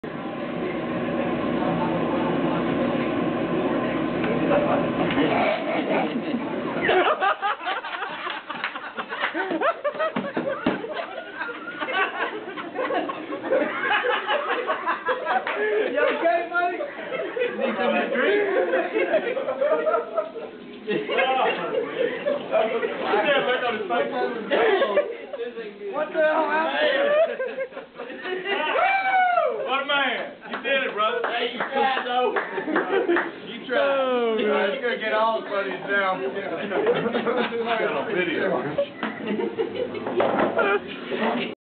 The cinnamon dragon
A rookie firefighter was challenged to eat a table spoon full of cinnamon. Thus the cinnamon dragon was born.